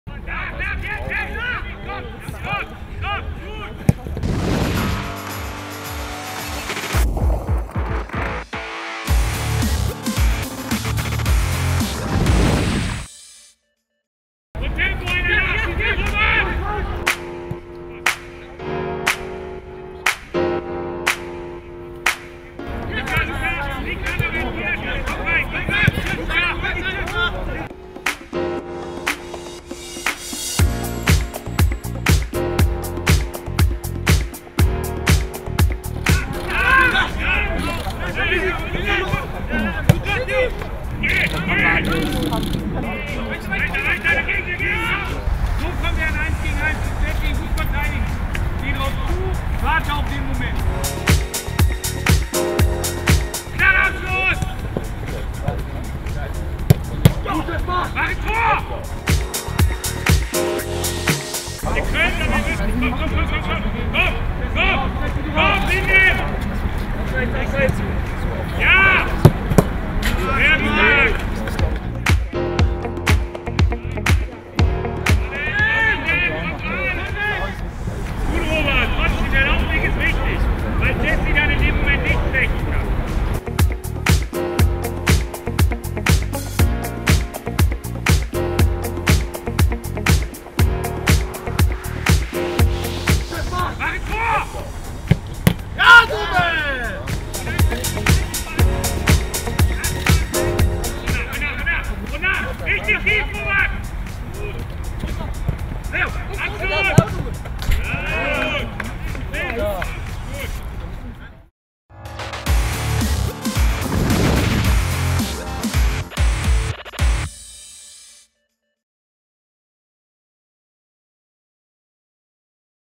Kohfeldt on fire!
Werder-Interimstrainer Florian Kohfeldt hatte sich bei den Trainingseinheiten am Dienstag mächtig ins Zeug gelegt - und gab immer wieder klare Anweisungen an seine Spieler.